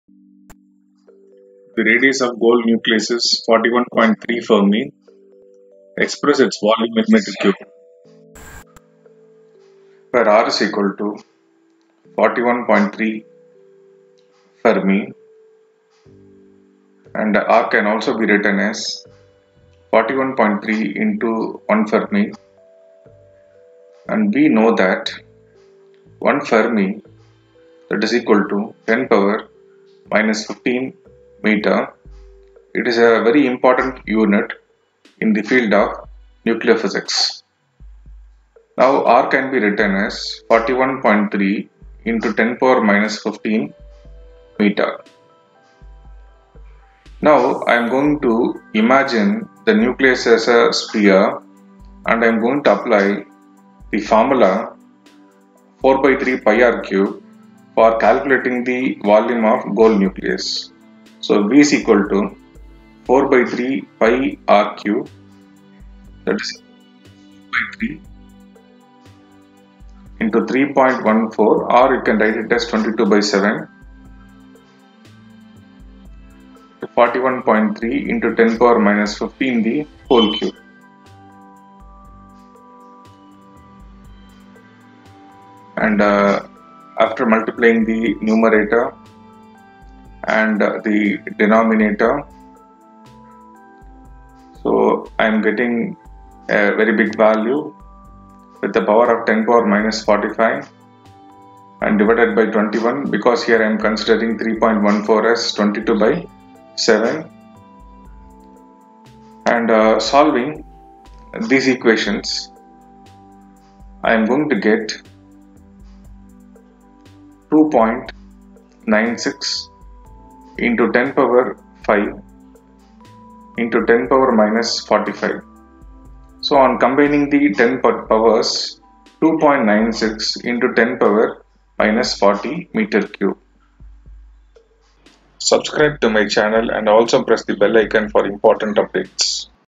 The radius of gold nucleus is 41.3fermi .Express its volume in metre cube
The radius of gold nucleus is 41.3 fermi. Express its volume in meter cube